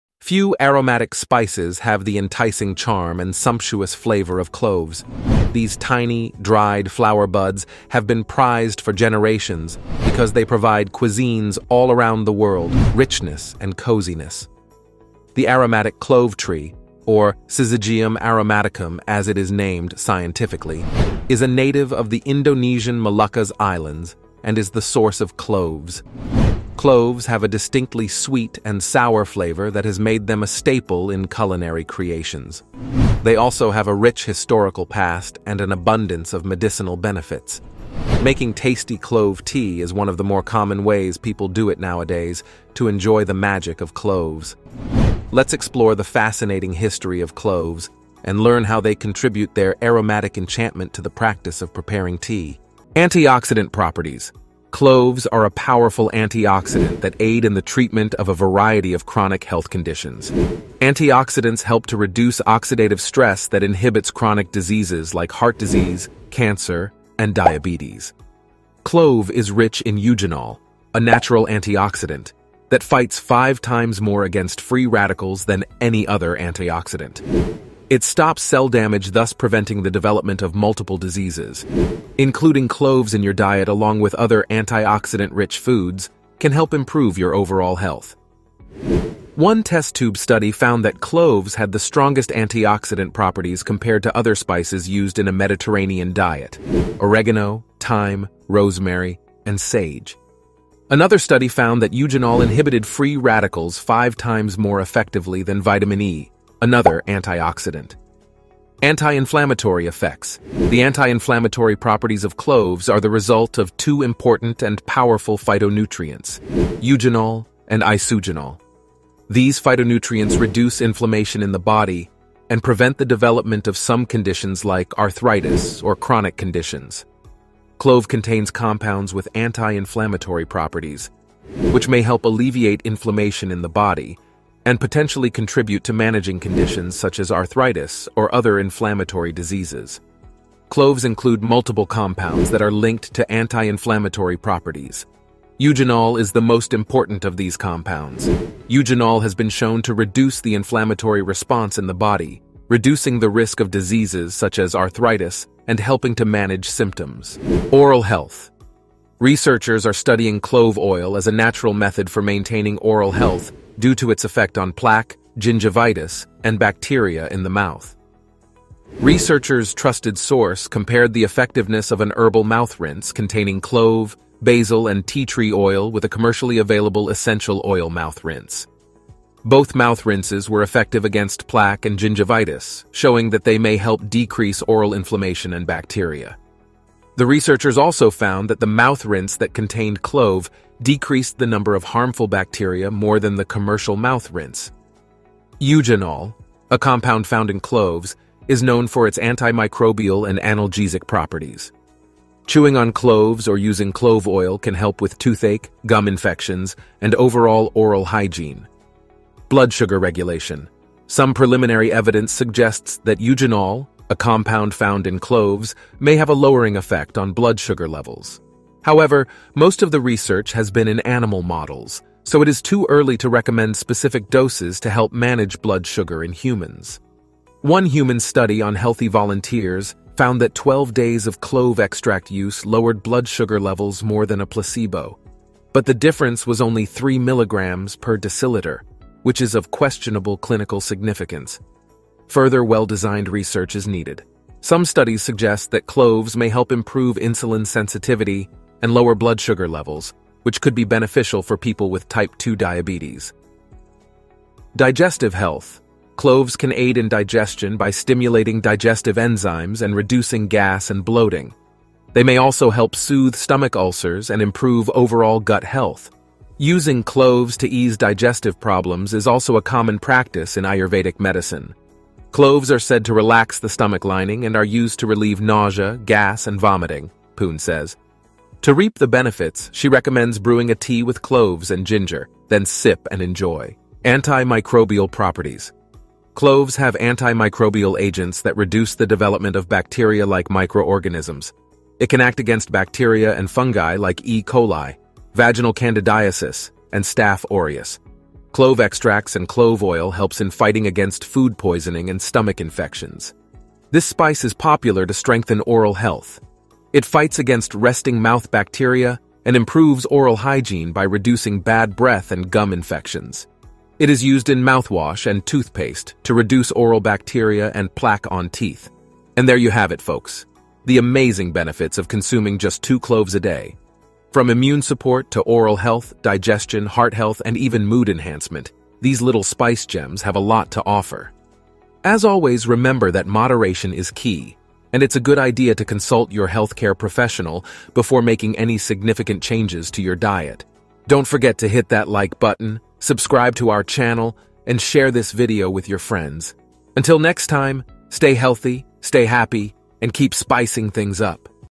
5 Reasons Why You Should Drink Clove Tea
5 Reasons Why You Should Drink Clove Tea

In today's video, we'll be discussing the incredible benefits of clove water at night. So, grab your favorite mug and join us as we uncover the healing properties of this magical elixir! Clove benefits, clove tea, clove tea benefits, drinking, tea lovers.

5 Reasons Why You Should Drink Clove Tea